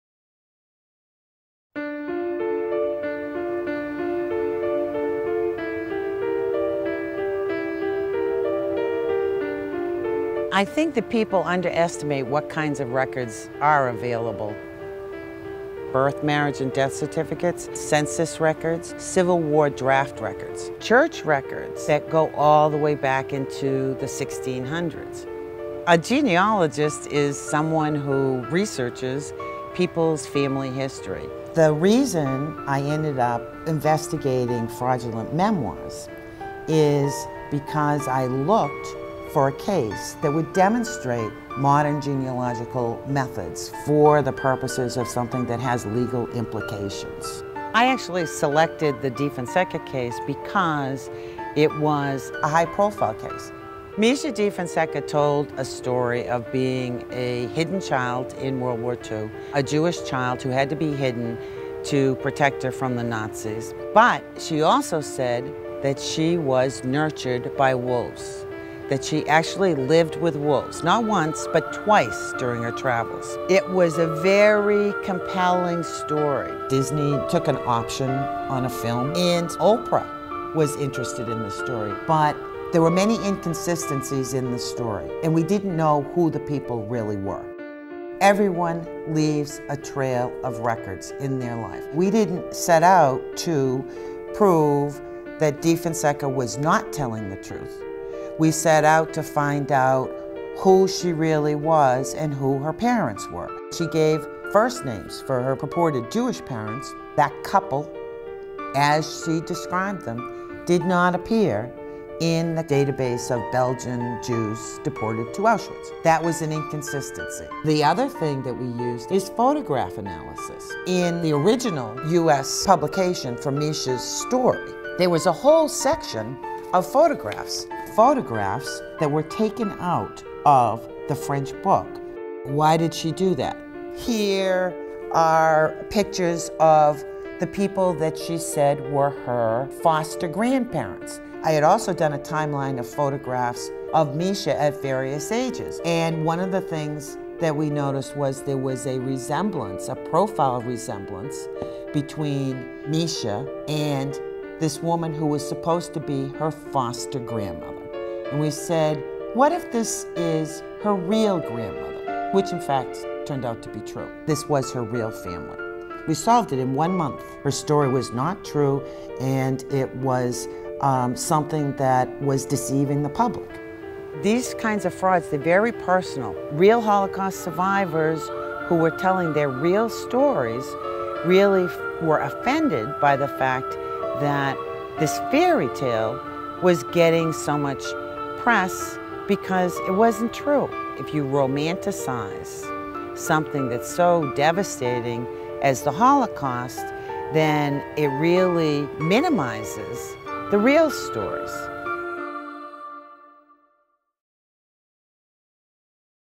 Hoax Busting
Sharon Sergeant has become widely known as a hoax buster, using forensic skills shes honed over 20 years to debunk three fraudulent Holocaust memoirs. Sergeants tools include photographic timelines and databases — vital records, census reports, property deeds, maps, newspaper interviews, obituaries, phone directories — and living relatives. Skype, online records, and blogs have also broadened her reach; DNA testing is an option if she needs it.